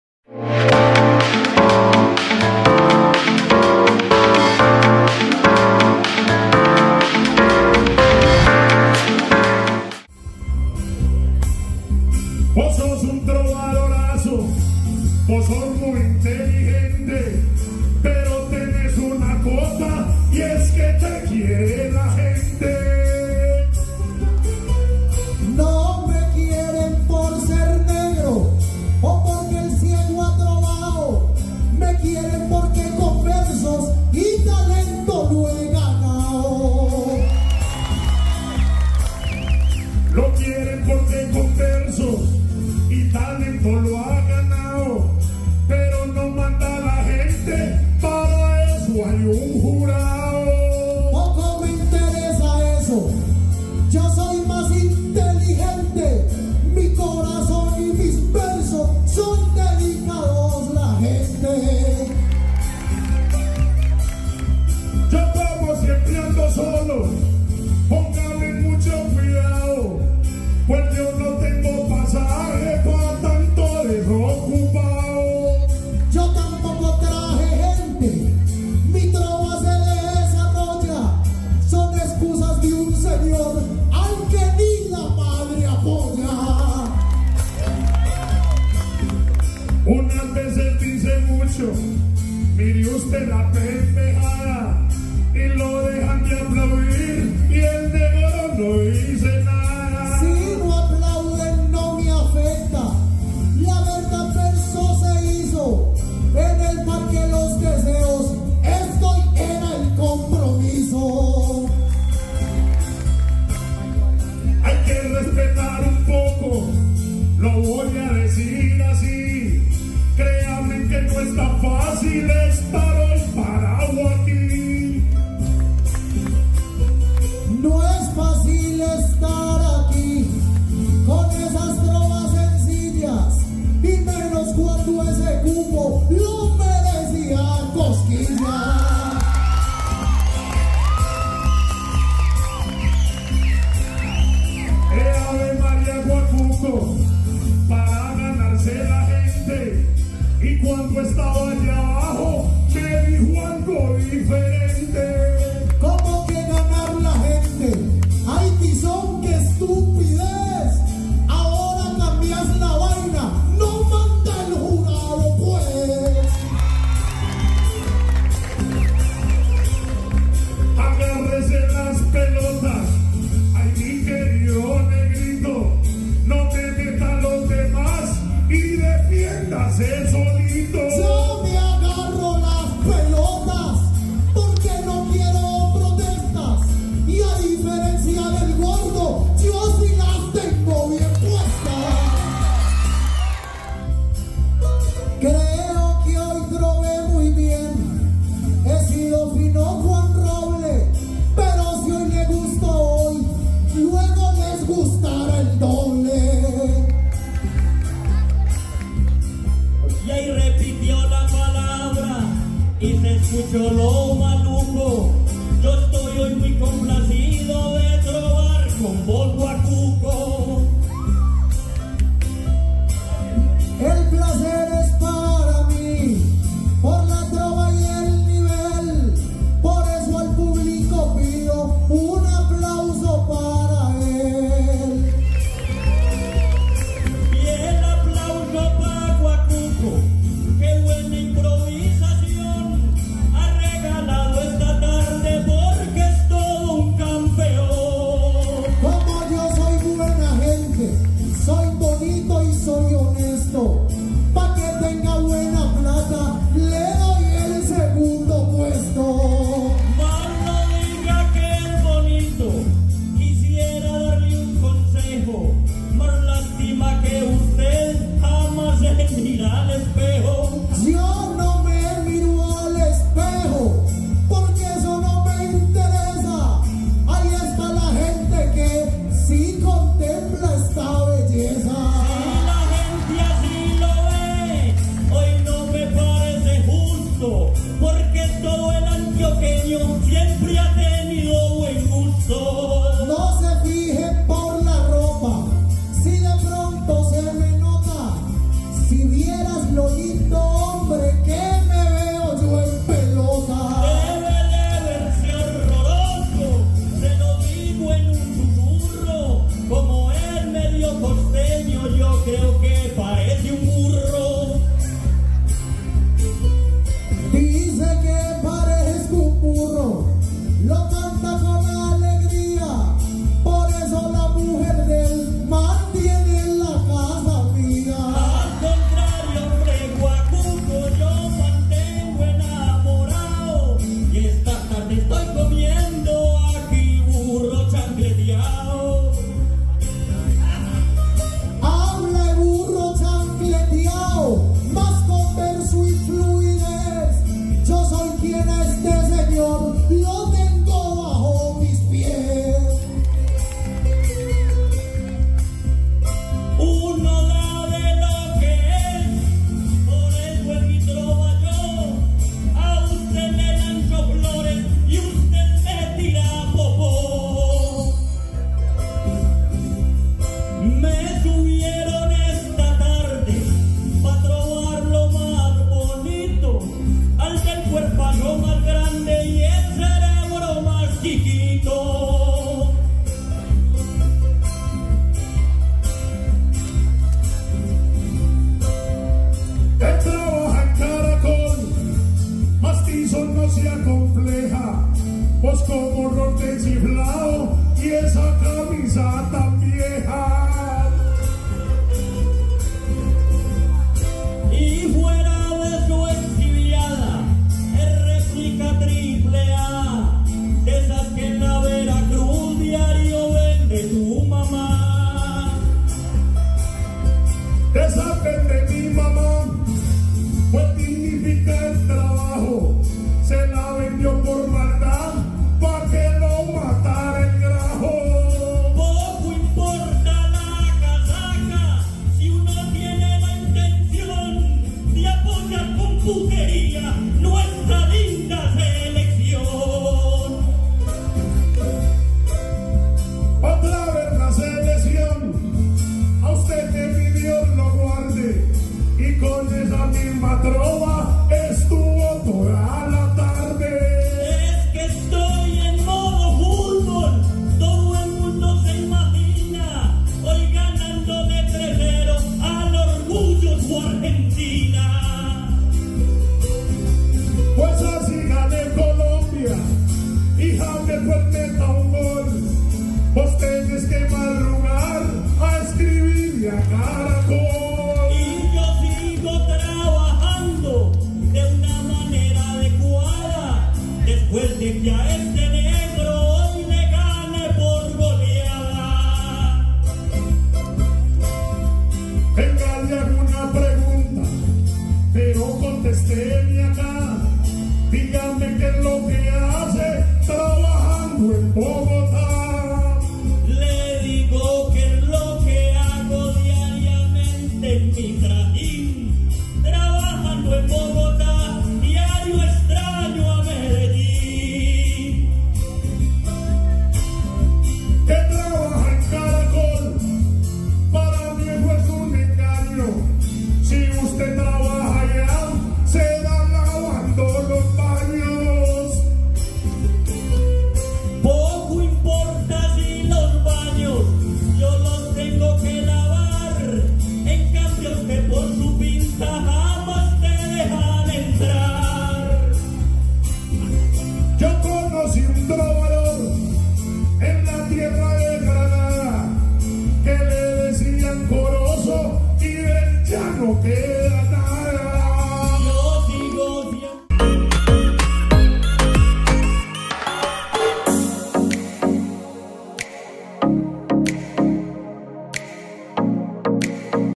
FESTIVAL DE LA TROVA CUIDAD DE MEDELLÍN 2024, Cuarta Clasificatoria, Ronda del Diablo, Tema Libre.
CuartaClasificatoria - Festival Nacional de la Trova Cuidad de Medellín 2024.
Pueblito Paisa - Medellín Antioquia.
Domingo 14 de julio de 2024.
Ronda del Diablo, Tema Libre, Completo.

Esta Jornada otorgó 3 cupos a la semifinal del Festival Nacional de la Trova Cuidad de Medellín 2024.

Rey: Tizón.
Virrey: Corozo.
Príncipe: Guacuco.

El cuarto puesto fue para Cosquillas, quien quedó como suplente para la semifinal de este festival.